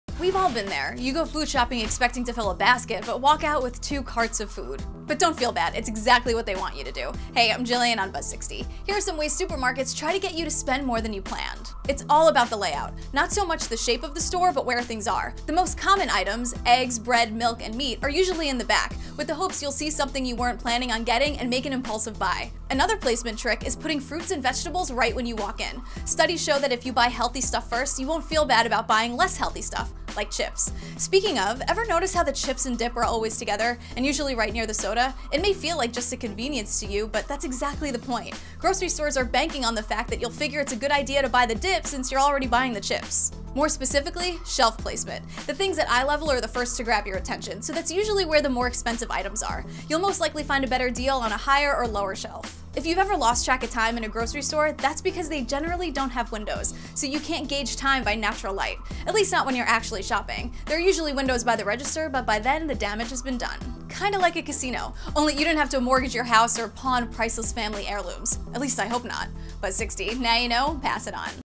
5 Ways Grocery Stores Trick You Into Spending More Money
Gillian Pensavalle (@GillianWithaG) tells what to keep in mind to save money when you go food shopping.